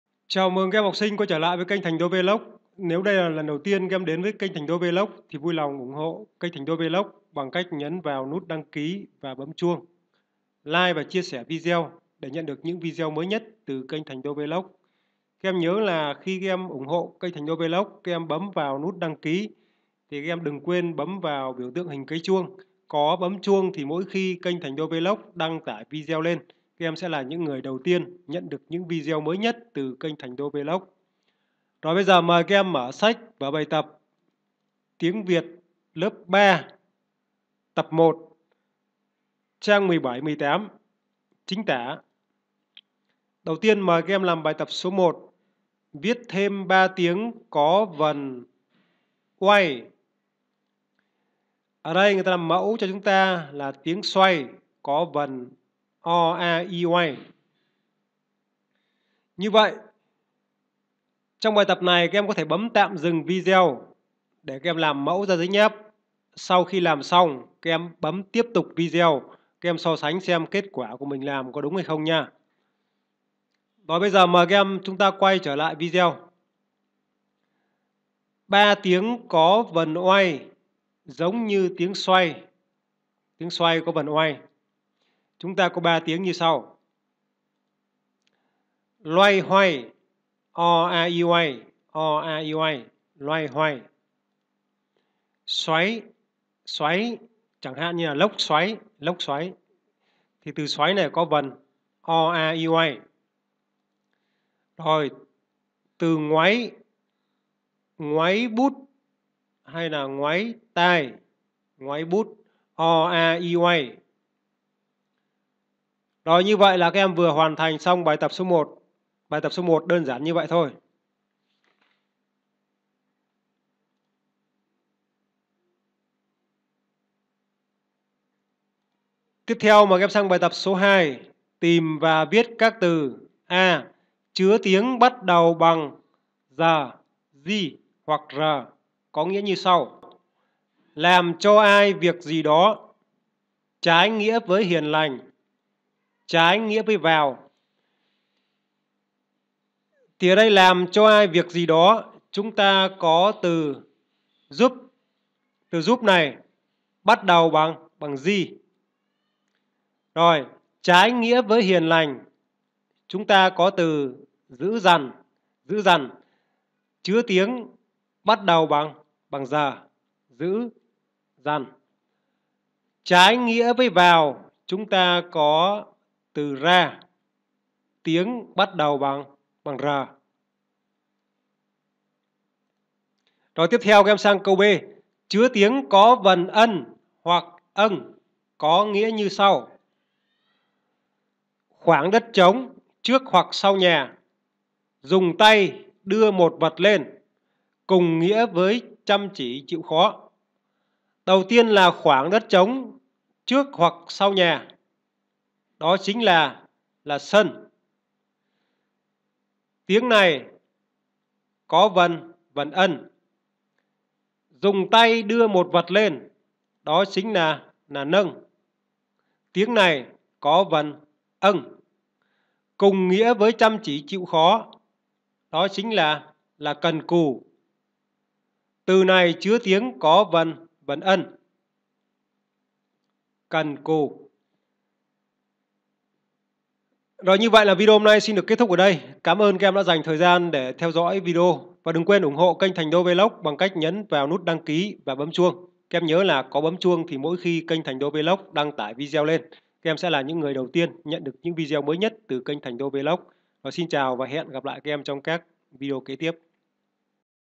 VỞ BÀI TẬP TIẾNG VIỆT LỚP 3 TẬP 1 TUẦN 4 CHÍNH TẢ TRANG 17, 18
1. Viết thêm 3 tiếng có vần oay :

M : xoay, loay hoay, (lốc) xoáy, ngoáy

2. Tìm và viết vào chỗ trống các từ :

a)  Chứa tiếng bắt đầu bằng d, gi hoặc r, nghĩa như sau :

– Làm cho ai việc gì đó :

– Trái nghĩa với hiền lành :

– Trái nghĩa với vào :

📣 Để nhận các tài liệu ôn thi, ôn tập, toán nâng cao, bài tập rèn luyện, đề thi tham khảo các em theo dõi trang trên Face book tại đây :